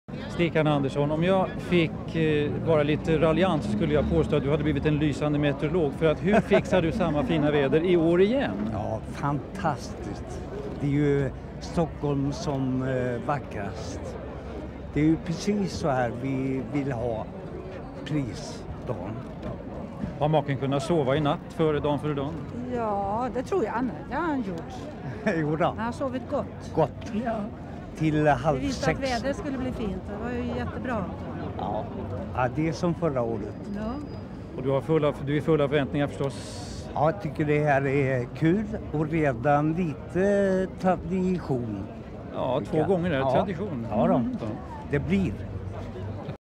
Interview - Founder of the Polar Music Prize Stig "Stikkan" Anderson
Interview with the swedish songwriter Stikkan Andersson - Polar Music Prize 1993.